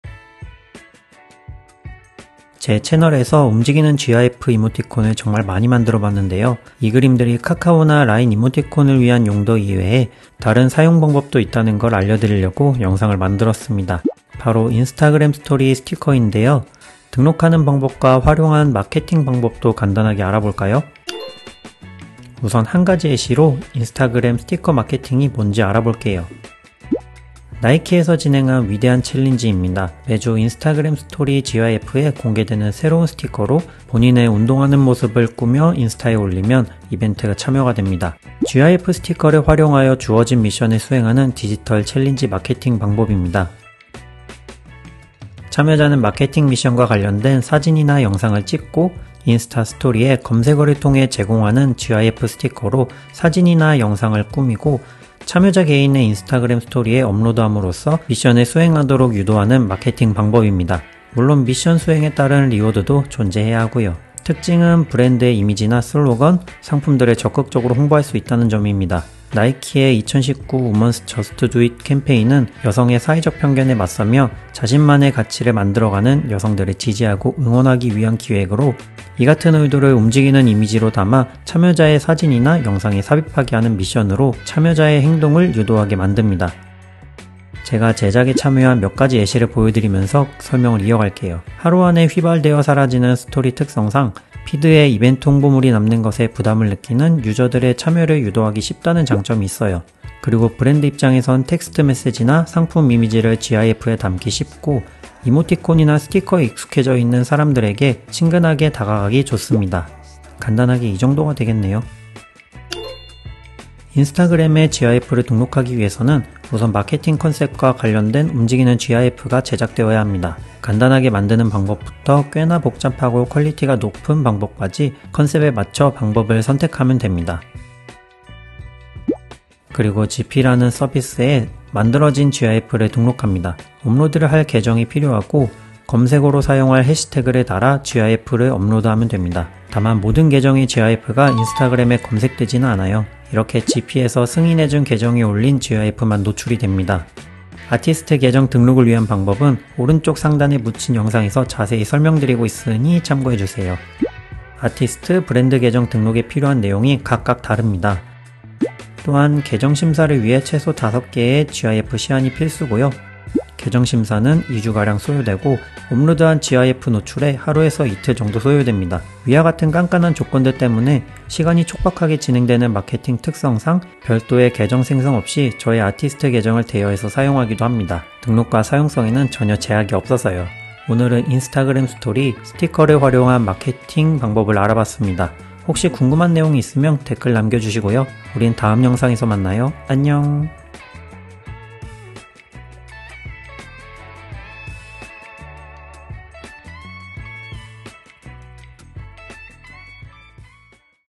인스타그램 스토리 스티커 마케팅? 등록방법까지!
GIF 스티커를 활용한 인스타그램 스토리 스티커 마케팅이 무엇인지
알아보도록 하겠습니다!
브릿지 사운드 : 류석원